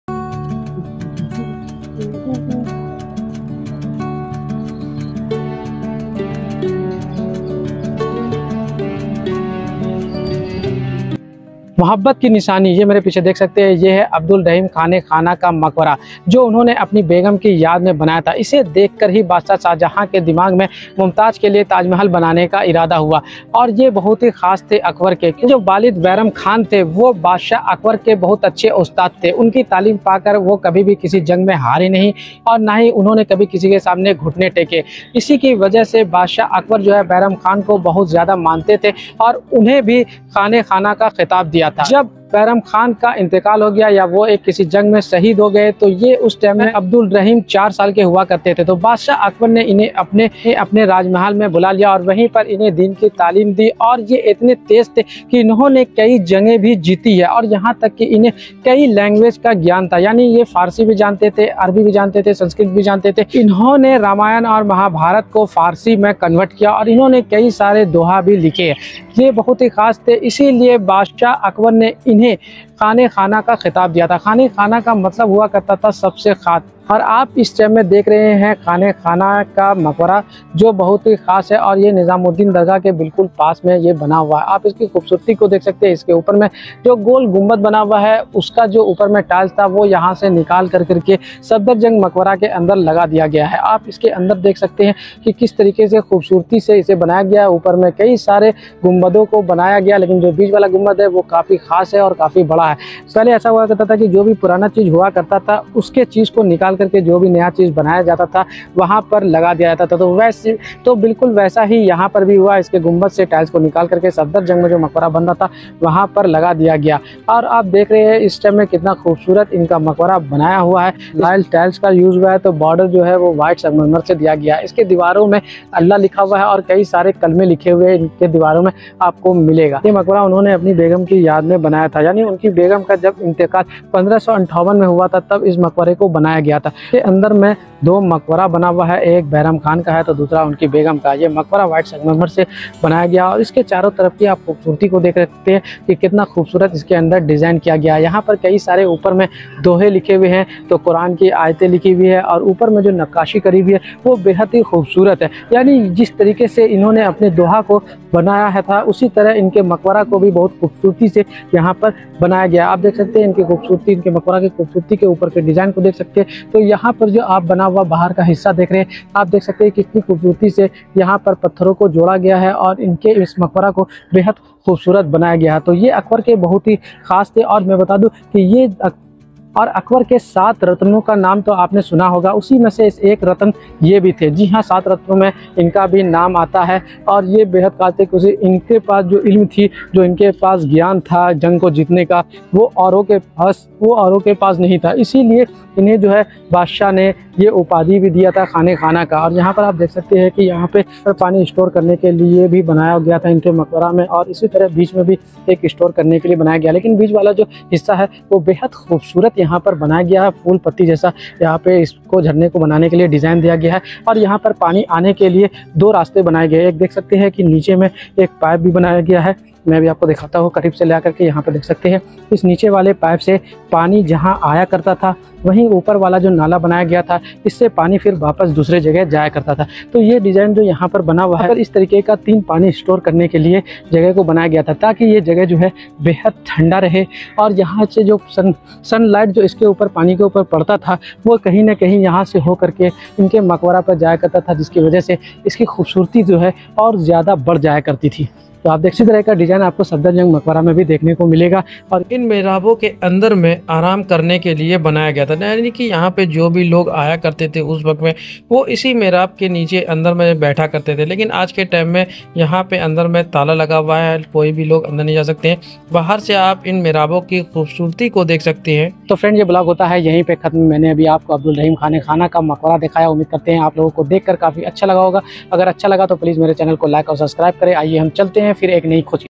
Delhi Ka Ek Anmol Ratan || Rahim Makbara || Akbar Ke Khas Ratan
Khanzada Mirza Khan Abdul Rahim (17 December 1556 – 1 October 1627), popularly known as simply Rahim and titled Khan-i-Khanan, was a poet who lived in India during the rule of Mughal emperor Akbar, who was Rahim's mentor. He was one of the nine important ministers (dewan) in Akbar's court, known as the Navaratnas. Rahim was known for his Hindustani dohe (couplets) and his books on astrology.

Note - please video ko pura dekhe Bahut mahnat se aap logo ke liye video banata hun,aap logo ki support ki jarurat hain..👏